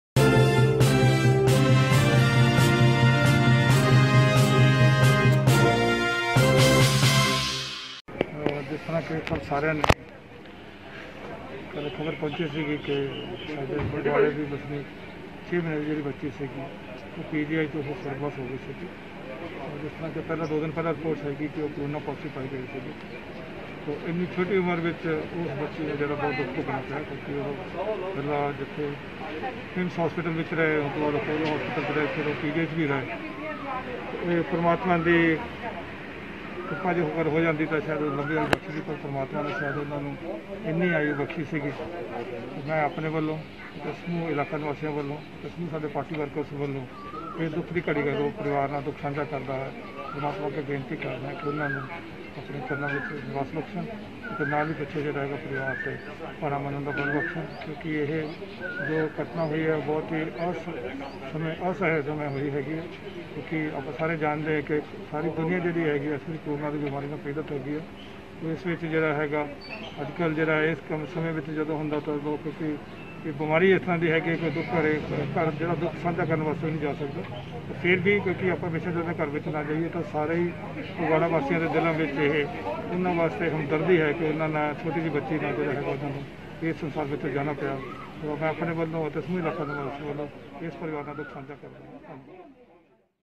ਐਮ ਐਲ ਏ ਬਲਵਿੰਦਰ ਸਿੰਘ ਧਾਲੀਵਾਲ ਨੇ ਬੱਚੀ ਦੀ ਮੌਤ ਤੇ ਕੀਤਾ ਦੁੱਖ ਦਾ ਪ੍ਰਗਟਾਵਾ
Follow with Youtube

Mobile App For Android